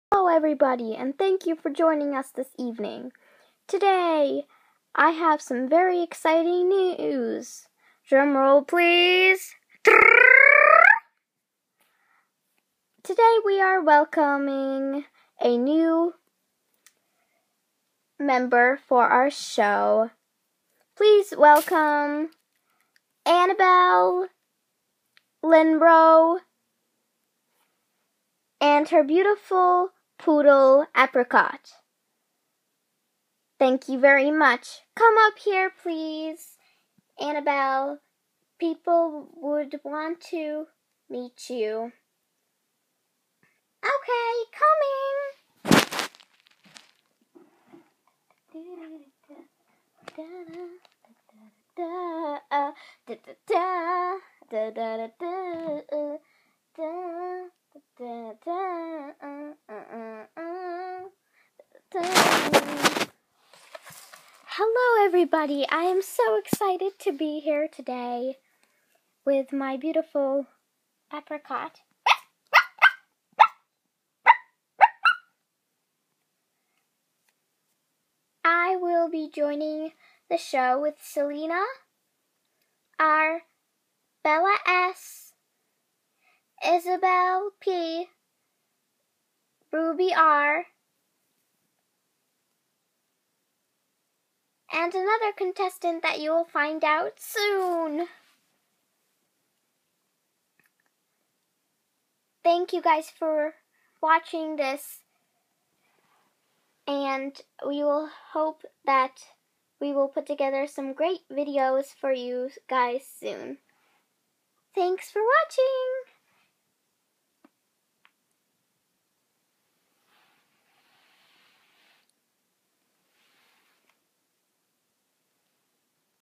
Meet our new member | new doll
I just got truly me #61 from the American girl doll store thank u for welcoming her today!